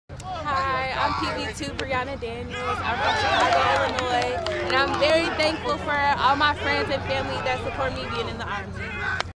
What I am Thankful For- PV2 Briana Daniels
Soldiers of the 428th Field Artillery participate in the 2022 Turkey Bowl and share what they are thankful for.

Fort Sill; Field Artillery; 428th FA; Thanksgivin